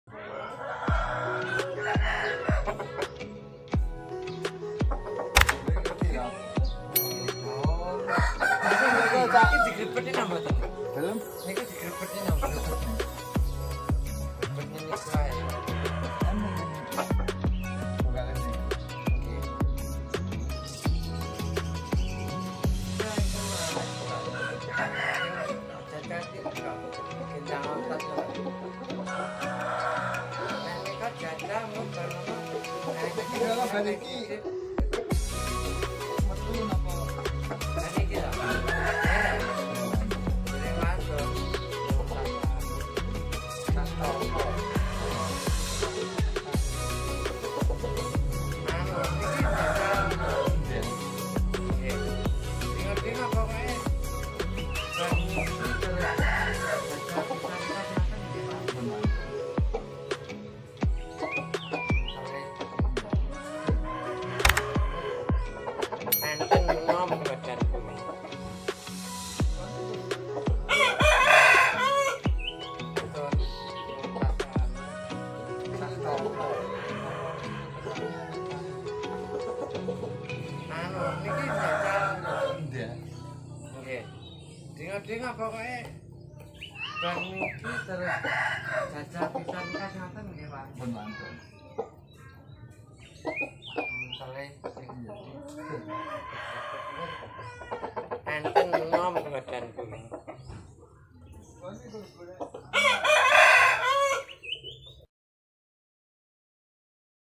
Ayam pama iq, pencetak anakan berkualitas jawara
Pama iq pejantan pilihan, penghasil anakan anakan aduan terbaik, dan banyak di minati pecinta ayam aduan istimewa teknik